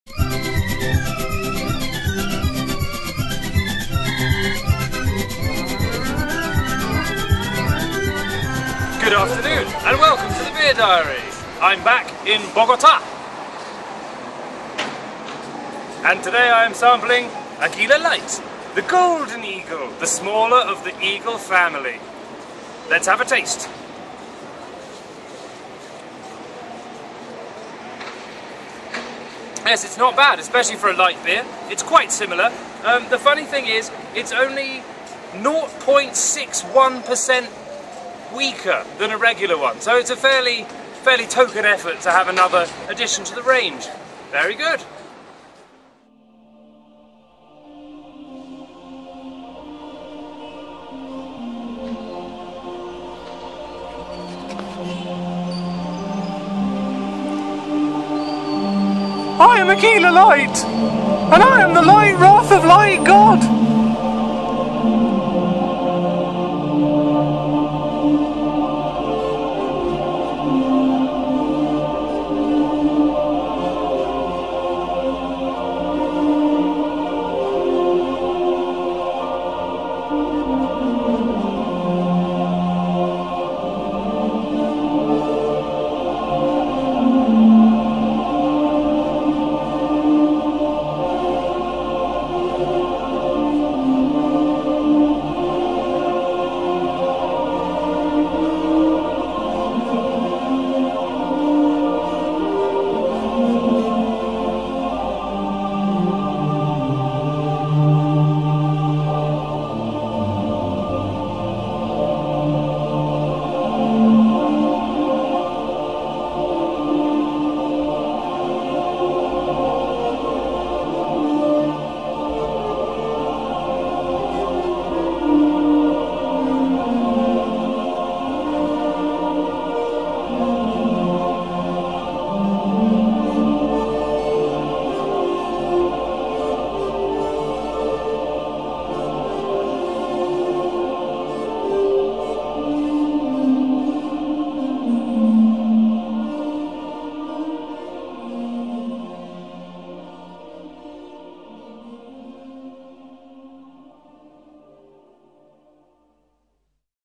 Beer Diary: Colombia (11)
Another week, another beer, another light lie. Donald Newholm natters whilst Lucy Newholm films.